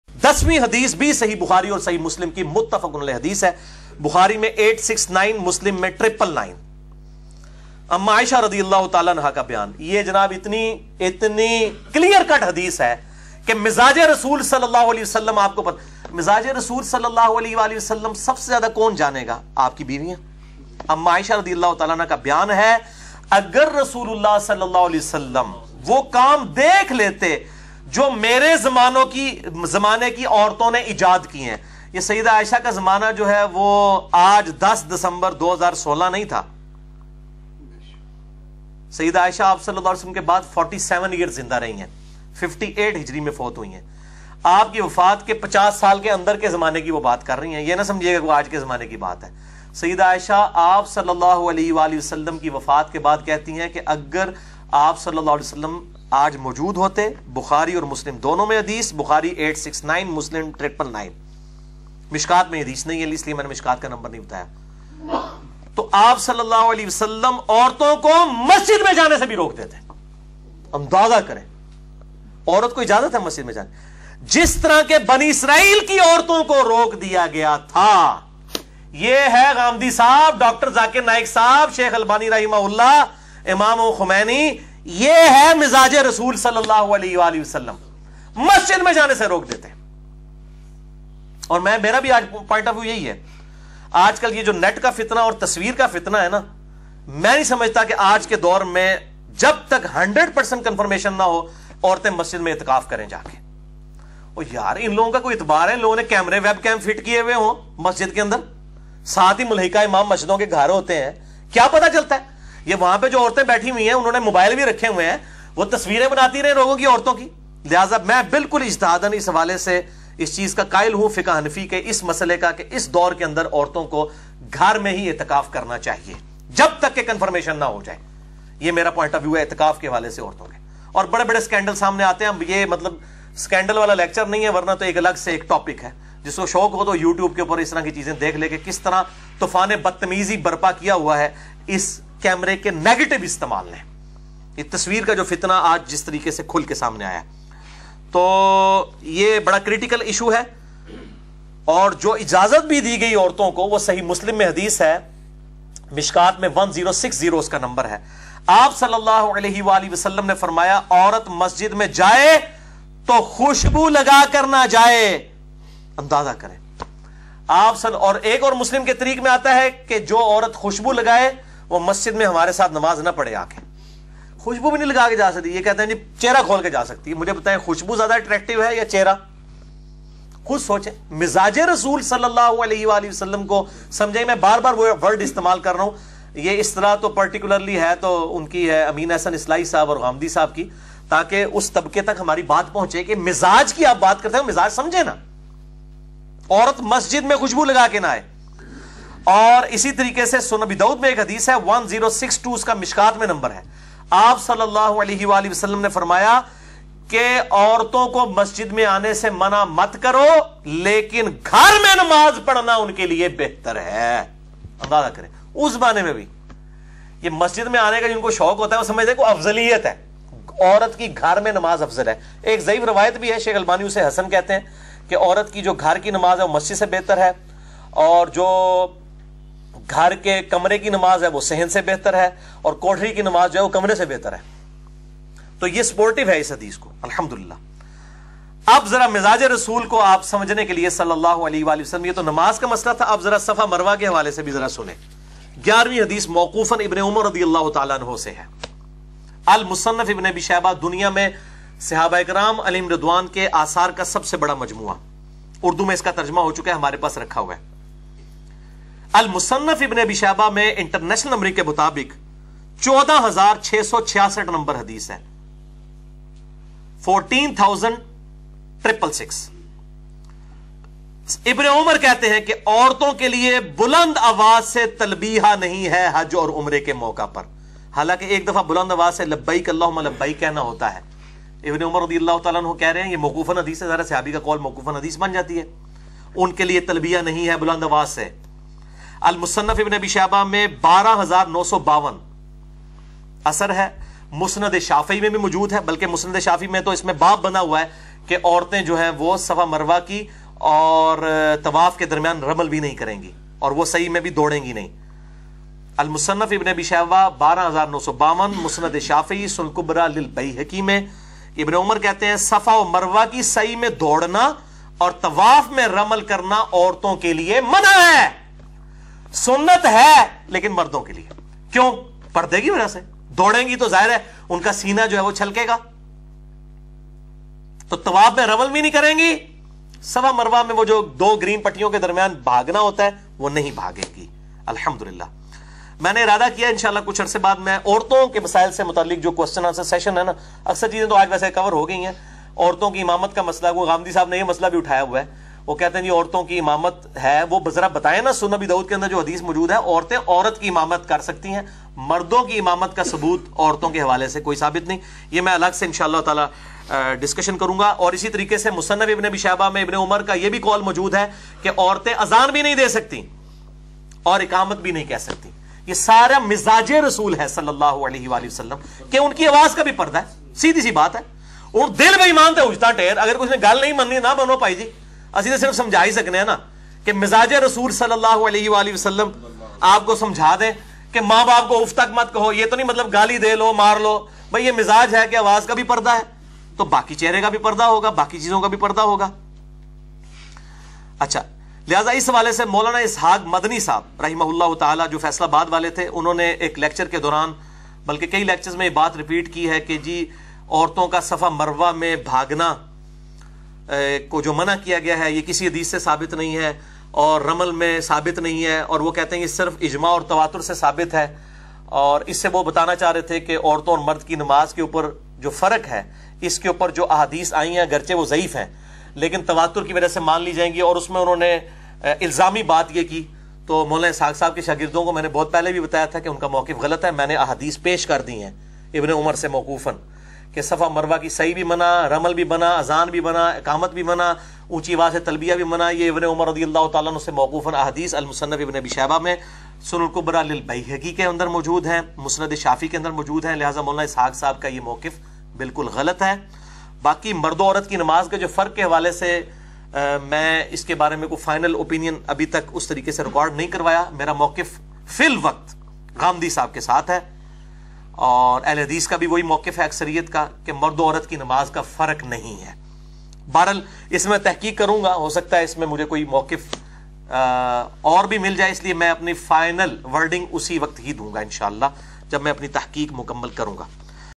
Kia MARD aur AURAT ki NAMAZ main koi FARAQ hai ??? Kia AURATAIN MASJD main NAMAZ parh sakti hain ???
A Video Clip from 161-b-Mas'alah By Engineer Muhammad Ali Mirza (Recorded on 10-Dec-2016). Link of Complete Lecture